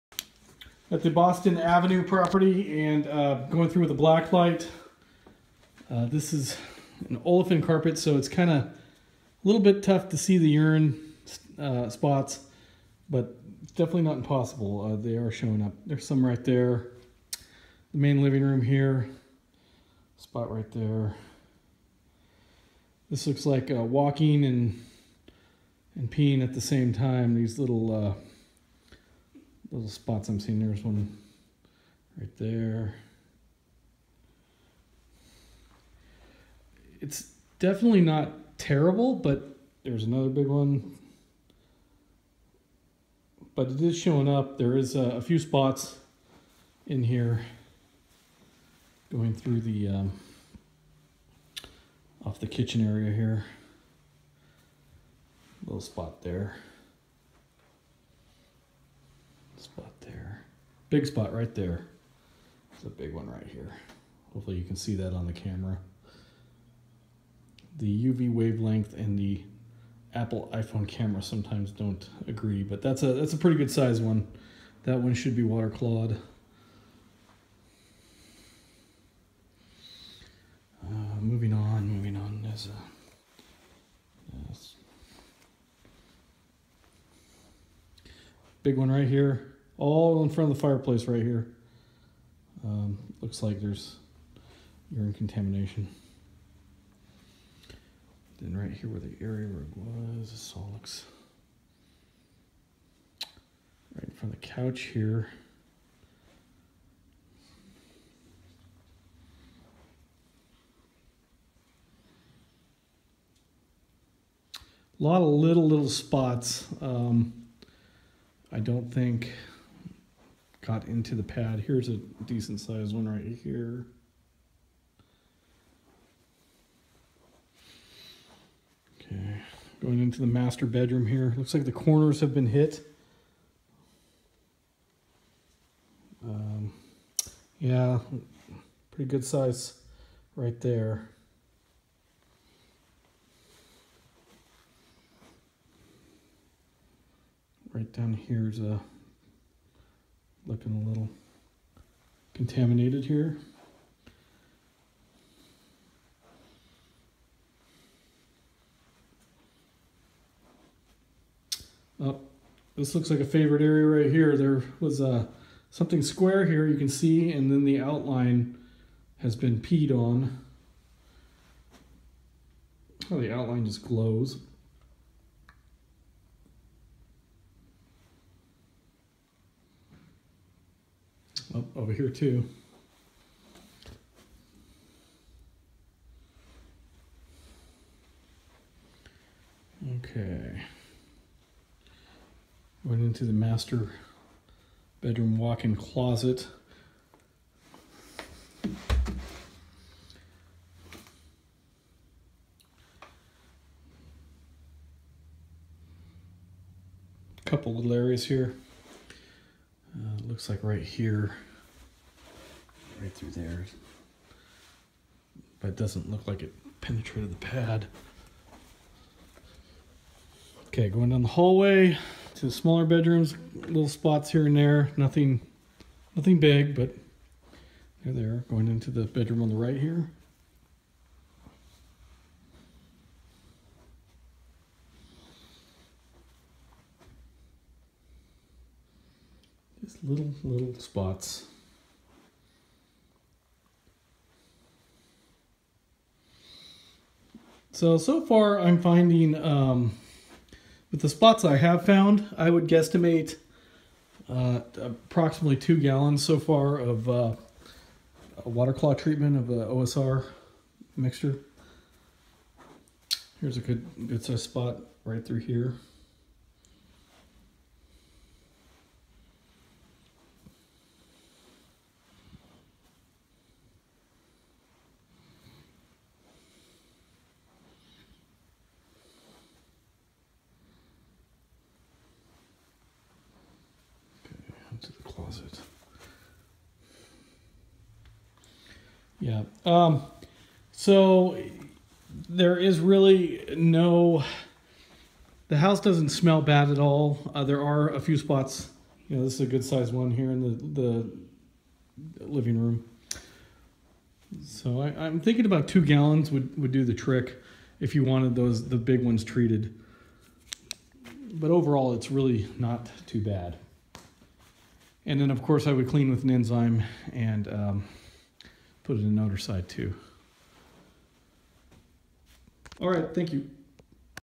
Boston Ave walk through
Black light inspection of urine contamination documentation.